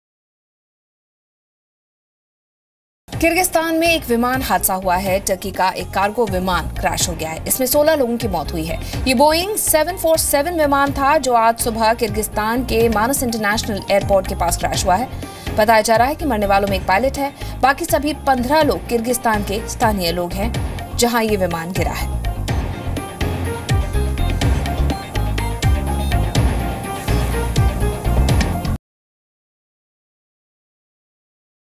Several dead after Turkish cargo plane crashes into Kyrgyzstan homes
Cargo airplane of Turkey crashed in Kirgistan, 16 people dead. Watch complete news story of News @ 9:00 AM to get the detailed news updates!